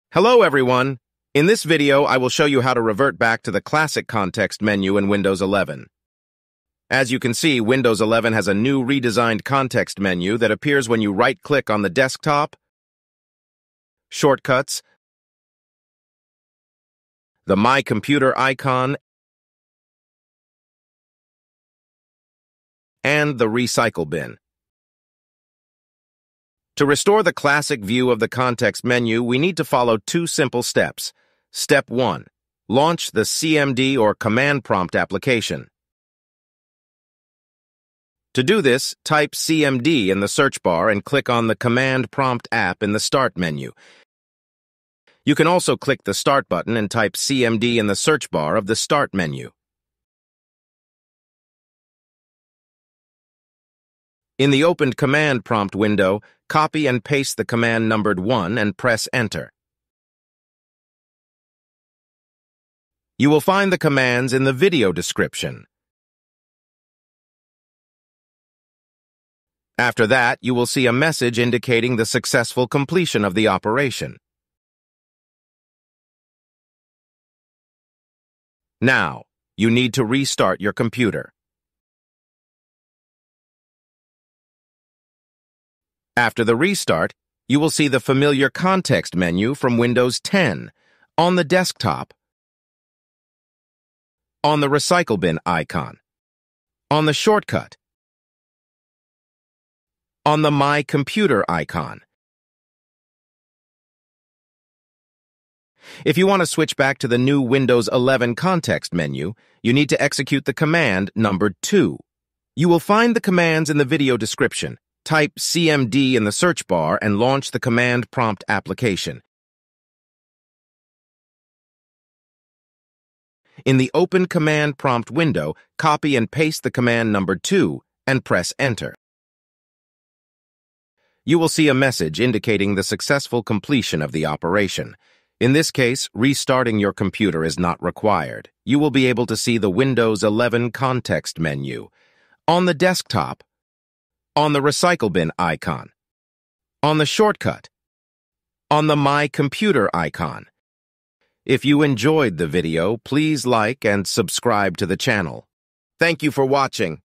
Windows 11: How to bring back classic Windows 10 right-click context menu
Hi all!
In this video, I will show you how to bring back to the classic right-click context menu (Windows 10 style) in Windows 11.

Table of contents:
00:00 Intro
00:12 New right-click context menu
00:30 Bring back old context menu
01:47 Switch back to the new context menu

1. Command to show old right-click menu (Windows 10 style):
reg.exe add "HKCU\Software\Classes\CLSID\{86ca1aa0-34aa-4e8b-a509-50c905bae2a2}\InprocServer32" /f /ve

2. Command to show new right-click menu  (Windows 11 style):
reg.exe delete "HKCU\Software\Classes\CLSID\{86ca1aa0-34aa-4e8b-a509-50c905bae2a2}" /f

To disable new context menu and enable classic context menu in windows 11, we need to follow two simple steps.

Step one:
Launch the "cmd" or Command Prompt application.
Step two:
In the opened Command Prompt window, copy and paste the command numbered 1 and press "Enter".
After the restart, you will see the old right-click menu from Windows 10.

If you want to switch back to the new Windows 11 context menu, you need to execute the command numbered 2.
Type "cmd" in the search bar and launch the Command Prompt application.
In the opened Command Prompt window, copy and paste the command numbered 2 and press Enter.
In this case, restarting your computer is not required.
You will be able to see the Windows 11 context menu.

❤️Did you enjoy my video?
Yours sincerely,
Just DIY.